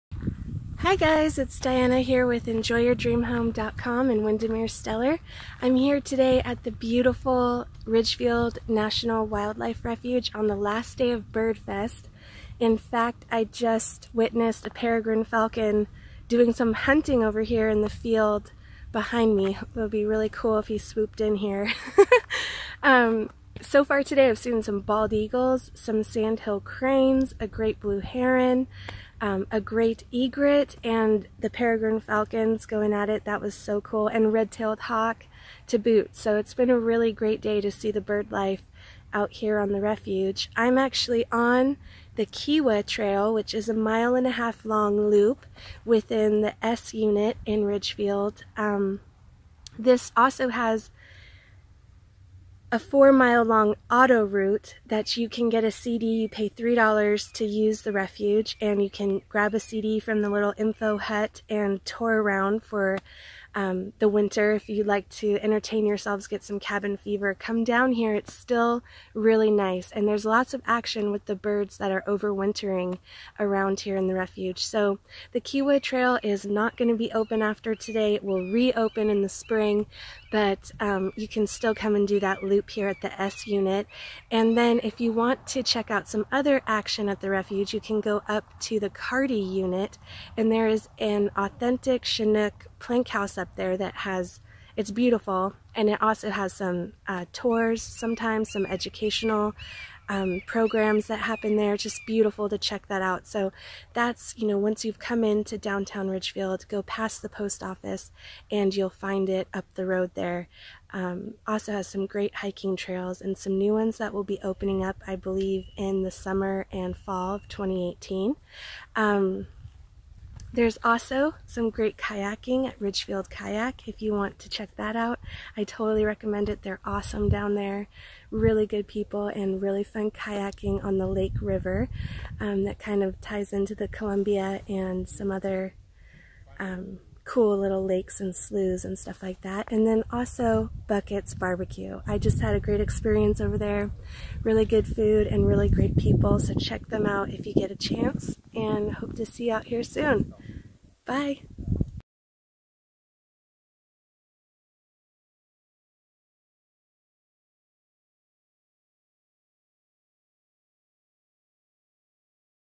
Ridgefield National Wildlife Refuge -Ridgefield, WA- Fall 2017
Watch this video for pictures and info about an authentic Chinook plankhouse-bald eagles-sparring peregrine falcons-gorgeous kayaking-world class photo opps- all found at the supremely beautiful Ridgefield National Wildlife Refuge, in Ridgefield,WA.

This refuge has something to offer everyone.  A beautiful sanctuary with walking trails, kayaking, historical interest, and excellent birding.  This is a real gem that can be enjoyed year round.  Come visit and find out why!